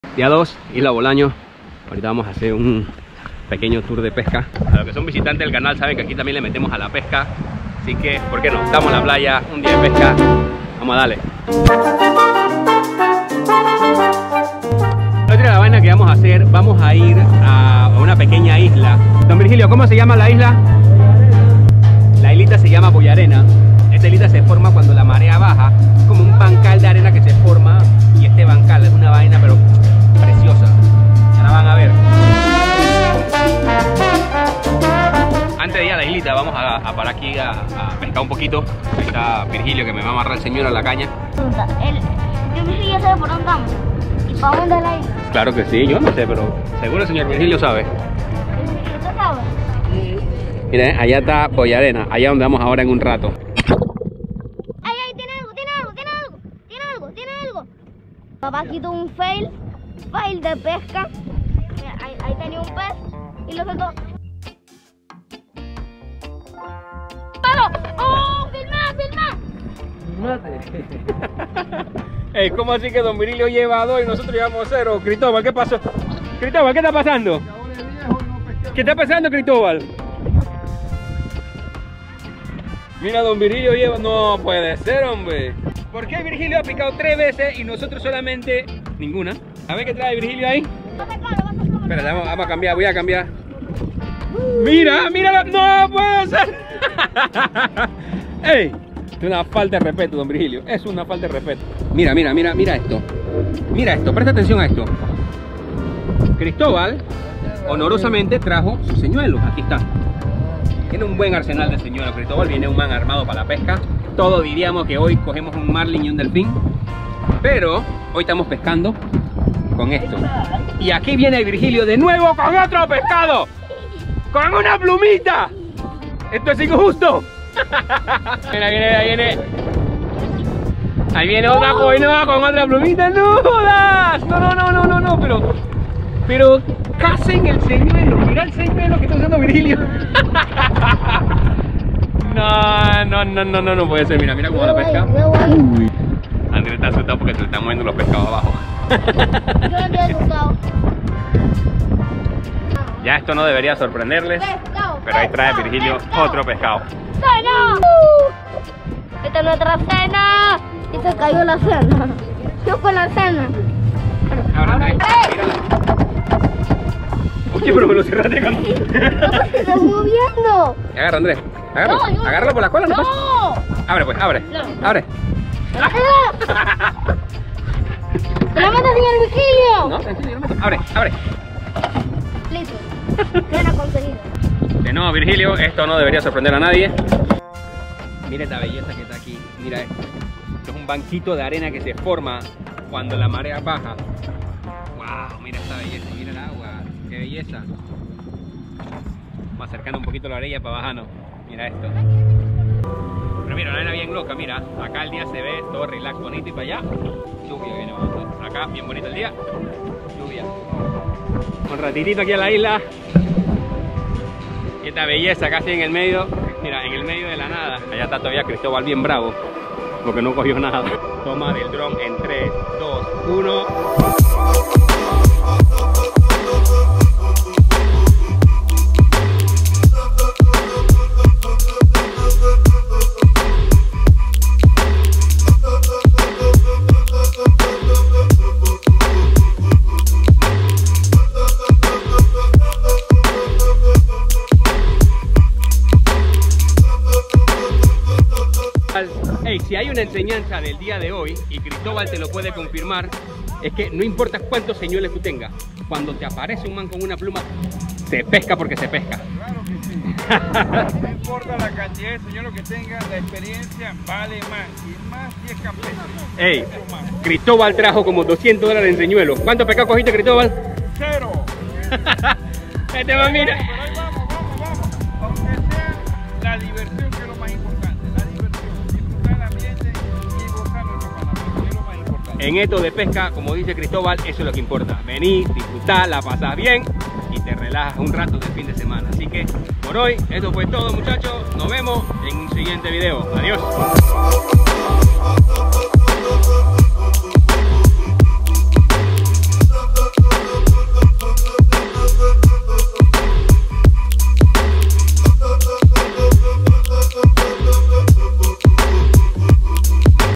FUIMOS DE PESCA Y A UNA ISLA EN EL MEDIO DEL MAR
Fuimos a pescar  a contadora y a una isla que se forma cuando baja la marea, se llama boya Arena. esta isla esta  en el archipielago de las perlas y es un paseo casi obligatorio si vas a isla bolanos.
todo el viaje fue con la gente de
ferrypearlislands.com.